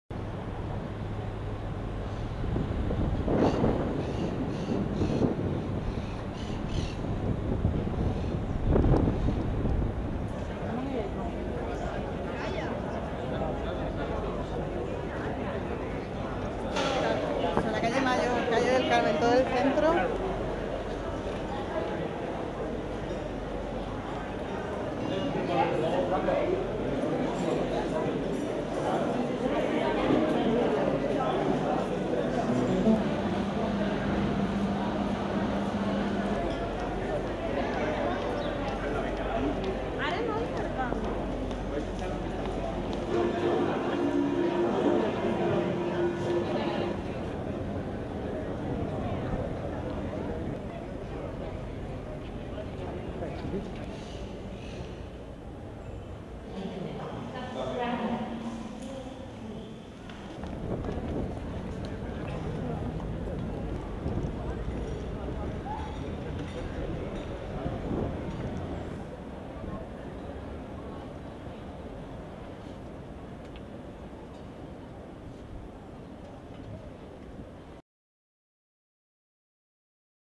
Más de 5 mil turistas hacen escala hoy en Cartagena
Cartagena volvía a llenarse de nuevo hoy de turistas que hacían escala en la ciudad. Aunque esta vez eran 3 cruceros los que recalaban en nuestra costa con un total de más de 5 mil pasajeros a bordo.

A primera hora, a las 8, llegaban el Noordam, de la compañía Holland America Line, con 1.918 pajaeros, y el Adventure of the Seas, del Royal Caribbean Cruises, con 3.144 pasajeros. Los cruceristas de ambas embarcaciones aprovechaban así toda la mañana para conocer a fondo Cartagena. Más tarde, a las 12, se sumaría el SeaDream II, del SeaDream Yacht Club, con 118 personas a bordo.

Hasta las seis de la tarde estos millares de turistas podrán disfrutar de la ciudad, así como de sus lugares más emblemáticos, el Museo del Teatro Romano, el Barrio del Foro Romano, el Augusteum o la recién reformada Casa de la Fortuna.

Además de conocer un poco más acerca de los 3 mil años de historia de nuestra ciudad, los turistas también degustaban las delicias gastronómicas típicas de la tierra como el caldero para acompañarlo después con el asiático. Los turistas, además, aprovechaban el buen tiempo que aún perdura desde verano para sentarse en alguna de las terrazas que hay por el centro de Cartagena, como las de la calle Mayor o la Plaza del Ayuntamiento.